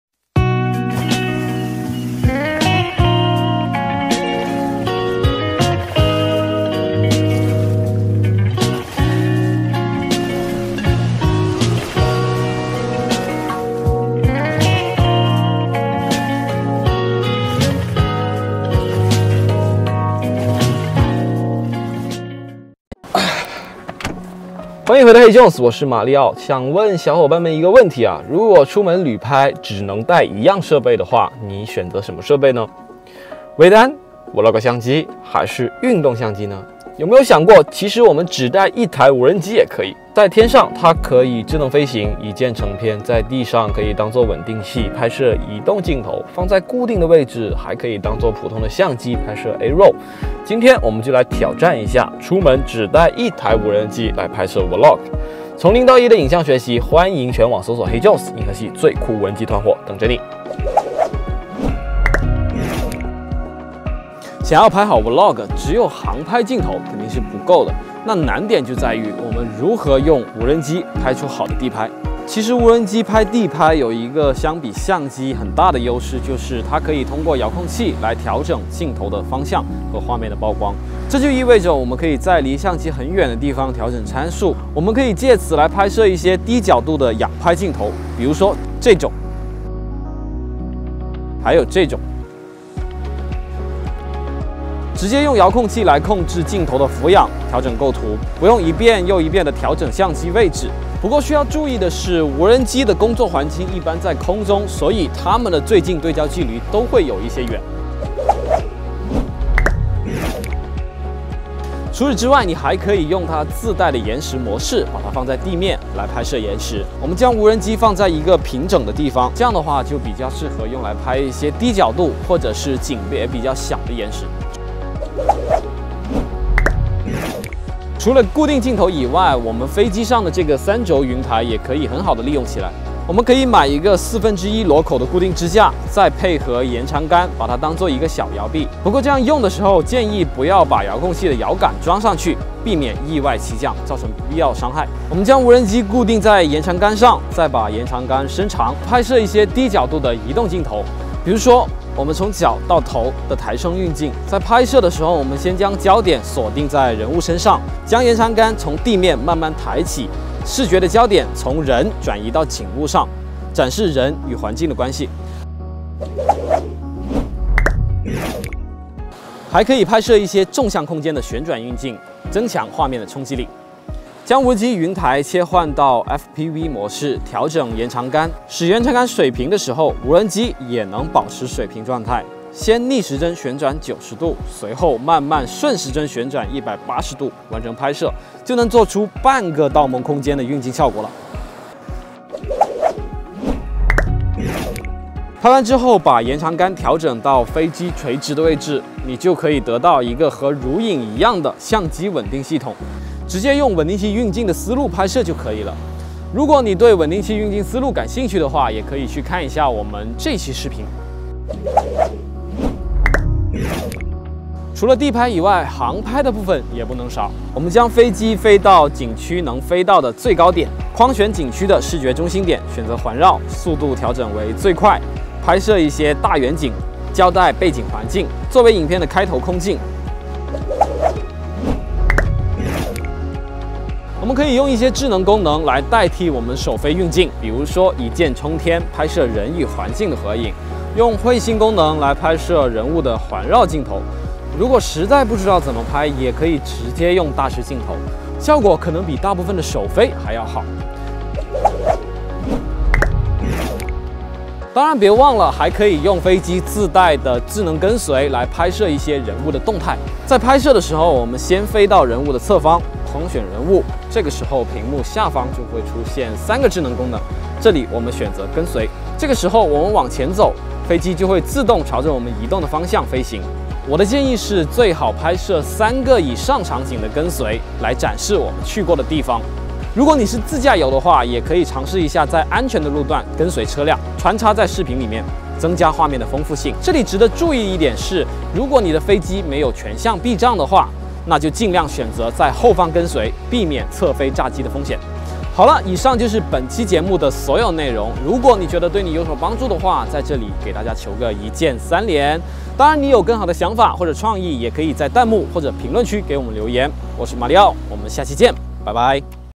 【干货】如何用1台无人机完成旅拍vlog拍摄？Mini3Pro的8种极限玩法！
挑战半天时间，用1台无人机进行旅拍vlog拍摄，我总结了这8种极限玩法，每一个都很干，快学起来！